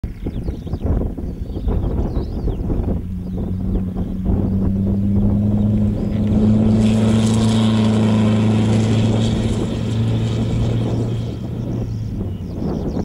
Баха Крым 2013 14
В Украине прошли очередную проверку железные колесницы и их владельцы, в этот раз им противостояла Крымская земля Творчесткое объединение "Koles.TV" представляет автомобильную программу "Koleso", а также обзор событий от друзей и партнеров.